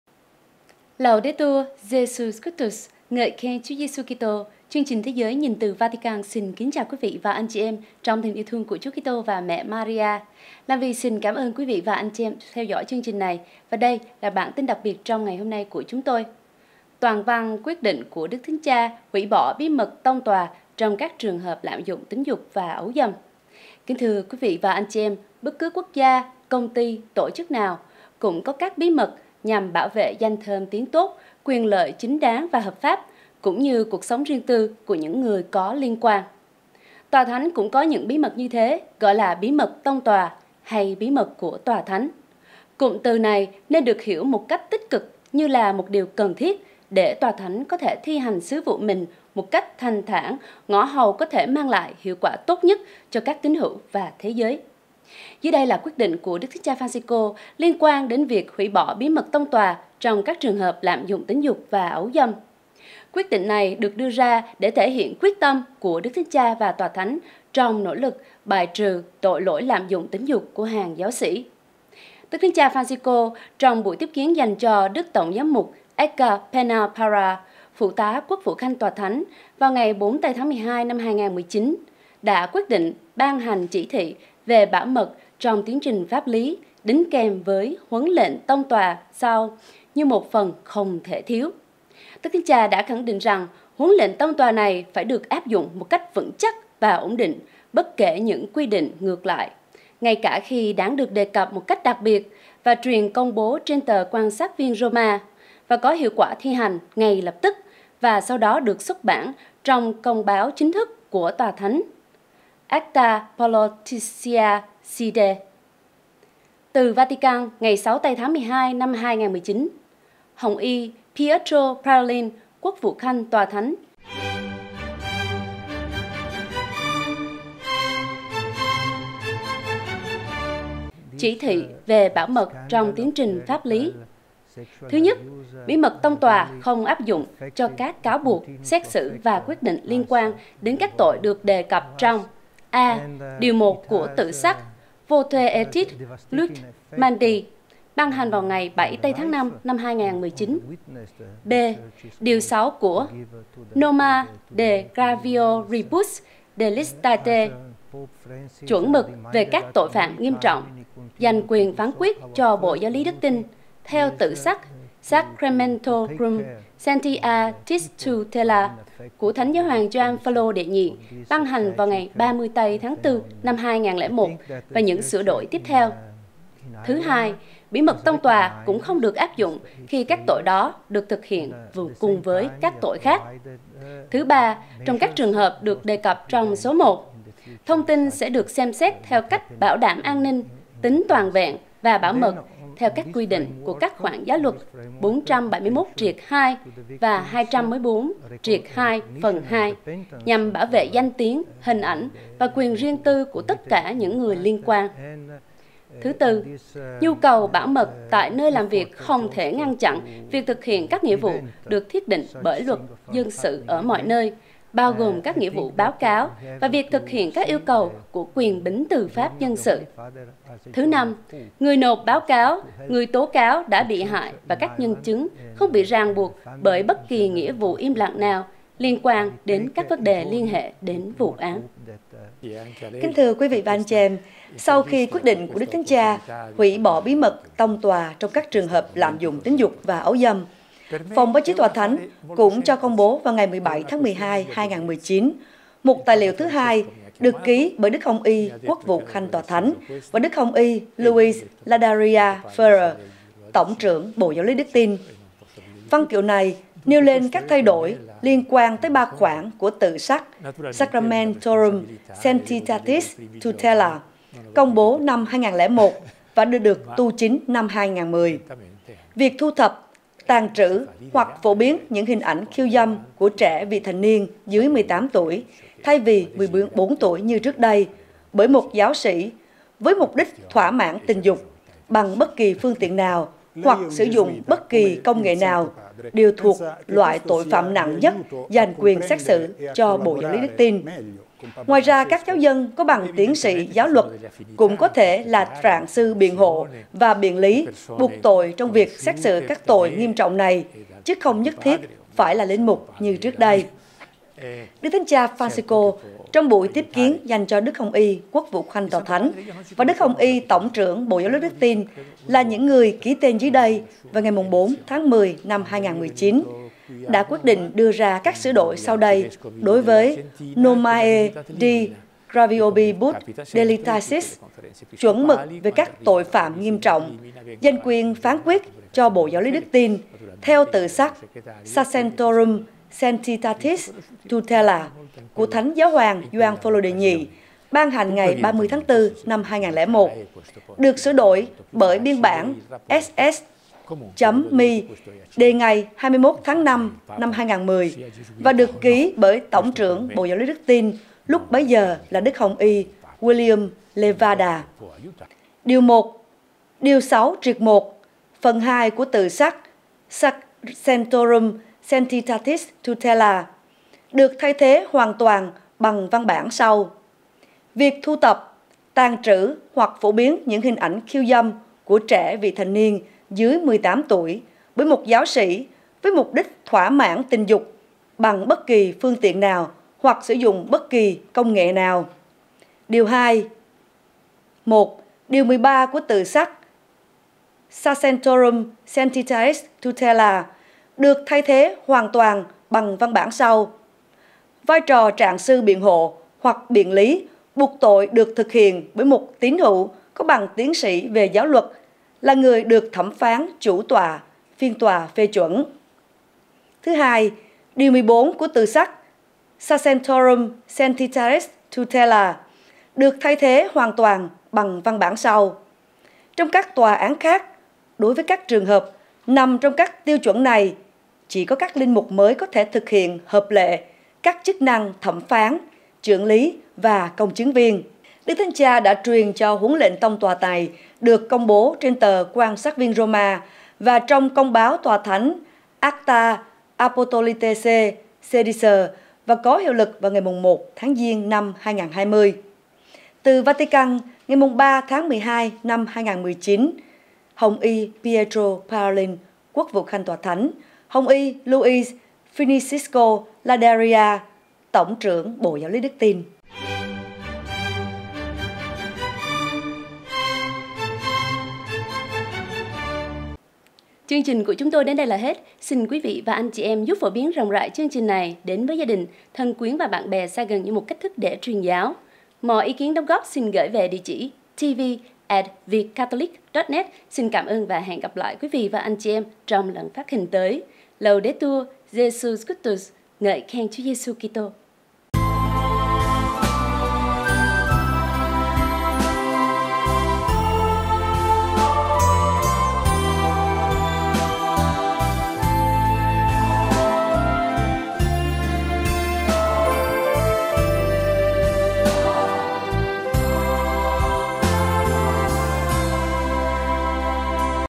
Bước quan trọng: Đức Thánh Cha hủy bỏ bí mật tông tòa trong trường hợp lạm dụng tính dục và ấu dâm
Quyết định của Đức Thánh Cha Phanxicô liên quan đến việc hủy bỏ bí mật tông tòa trong các trường hợp lạm dụng tính dục và ấu dâm được đưa ra để thể hiện quyết tâm của Đức Thánh Cha và Tòa Thánh trong nỗ lực bài trừ tội lỗi lạm dụng tính dục của hàng giáo sĩ.